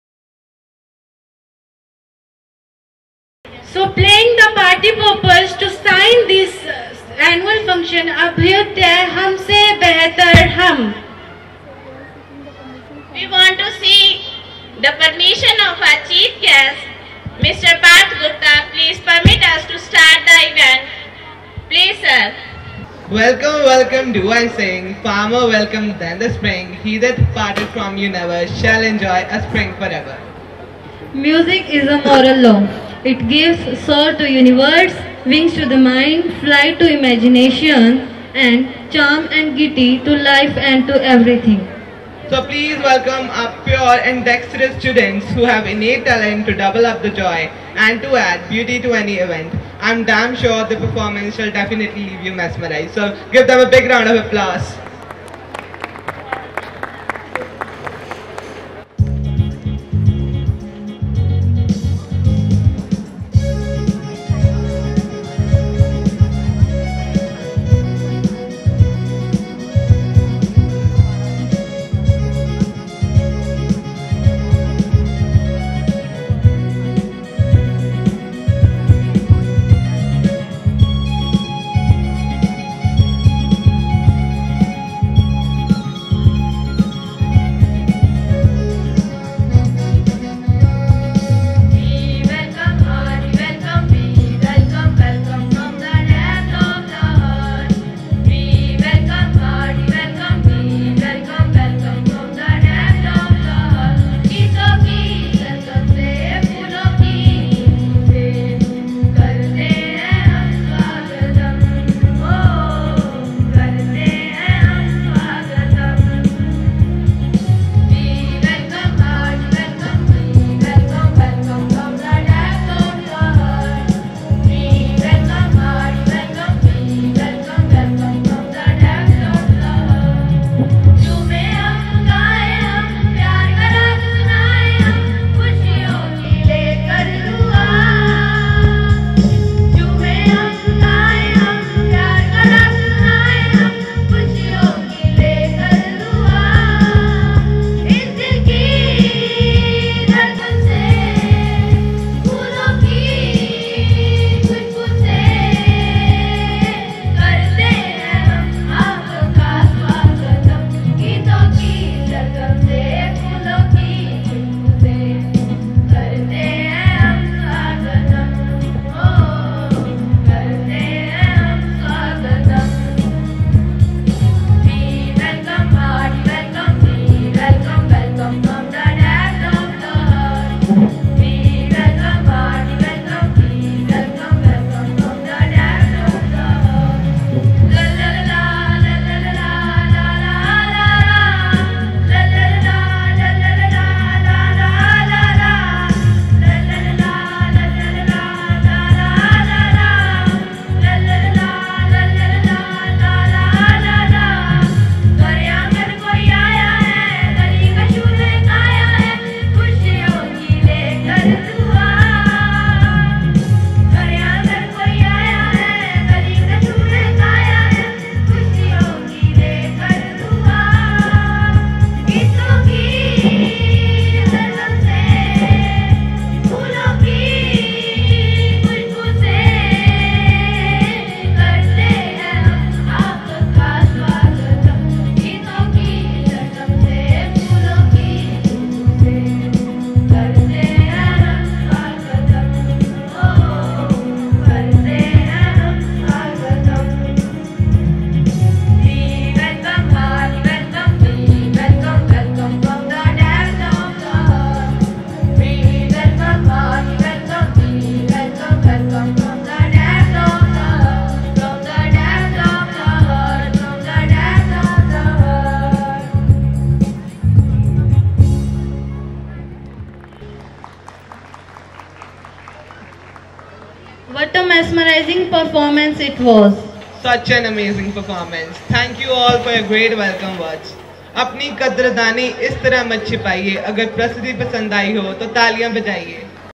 WELCOME SONG at अभ्युदय-“हम से बेहतर हम” RAJENDRA PUBLIC SCHOOL
Staff members and students of Rajendra Public School, a Panjuwana, Sirsa (Haryana) welcome you at अभ्युदय-“हम से बेहतर हम” with a precious song.